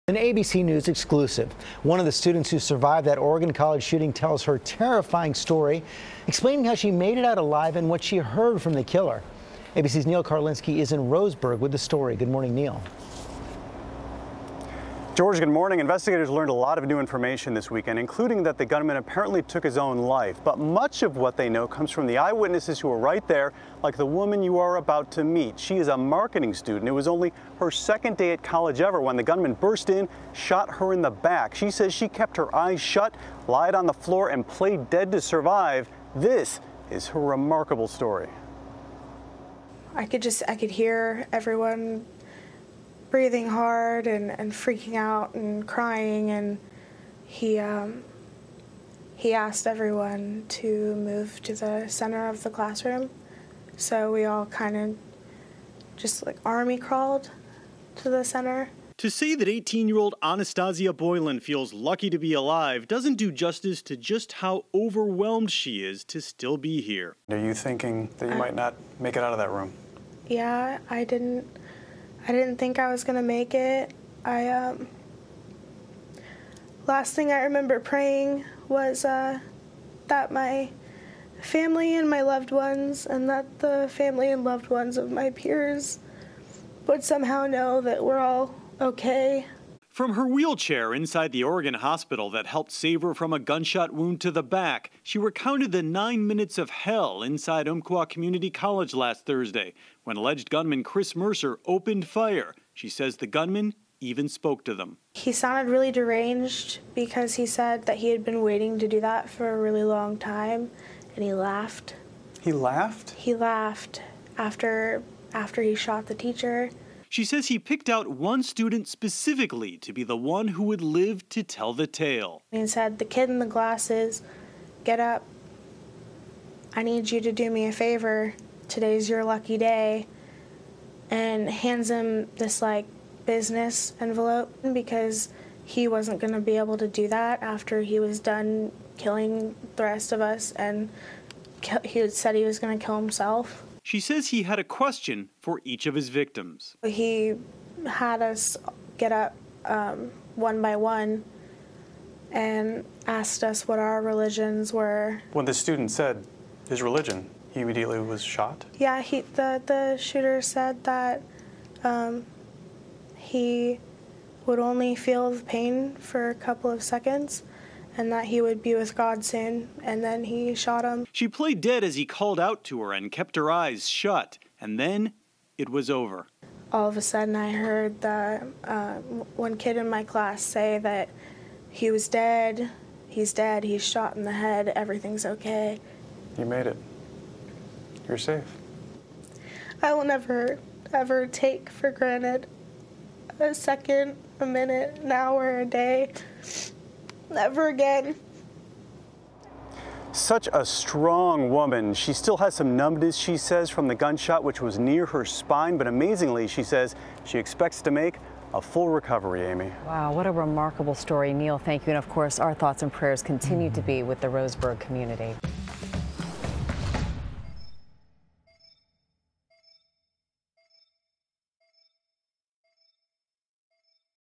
Oregon College Shooting: Survivor's Story Inside the Classroom
"I will never ever take for granted a second, a minute, an hour a day.." ‪#‎UCCShooting‬ survivor Anastasia Boylan discusses what happened inside Umpqua Community College after the alleged shooter Chris Mercer opened fire.